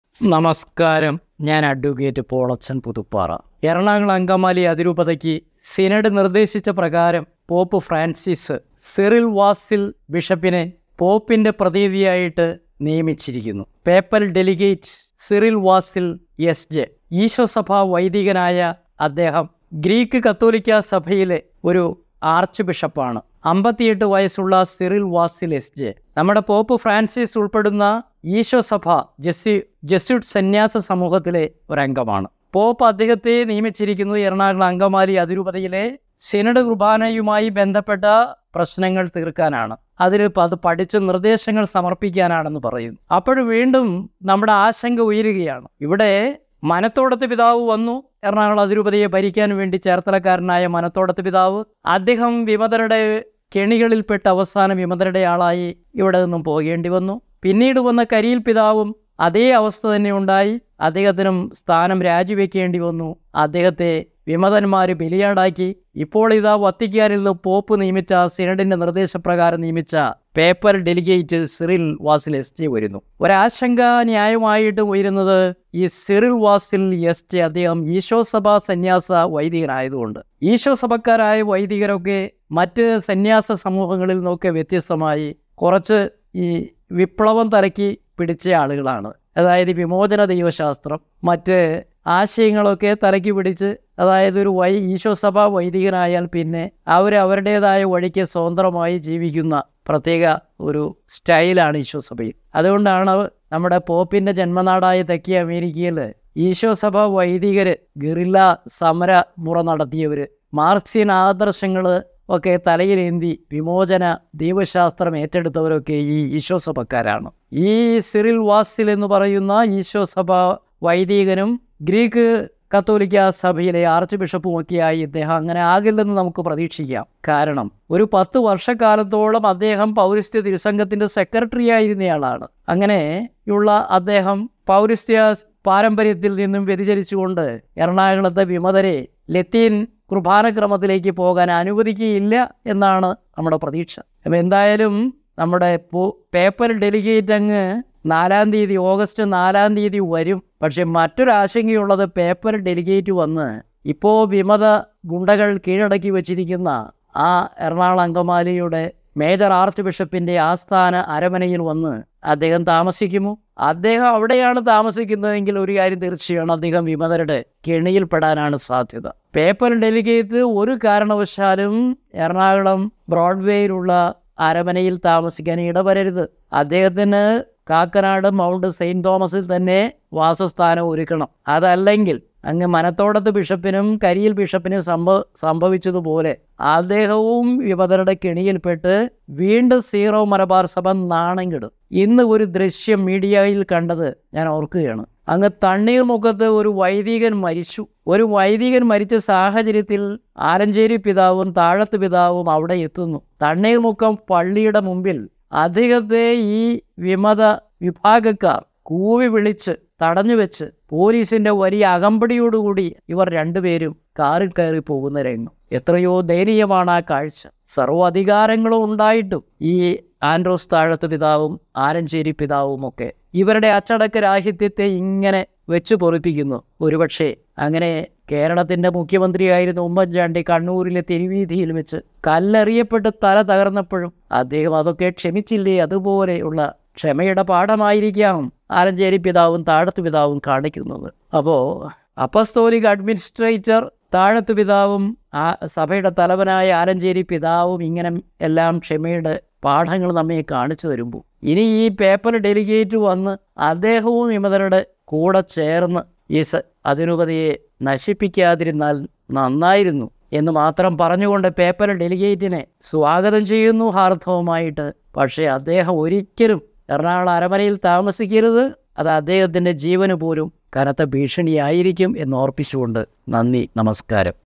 Arch bishop Cyril Vasil appointed as Papal Deligate in Ernakulam Angamaly Arch- diocese.
ആർച്ചു ബിഷപ്പ് സിറിൽ വാസിൽ എറണാകുളം അങ്കമാലി അതിരൂപത പേപ്പൽ ഡെലിഗേറ്റ്.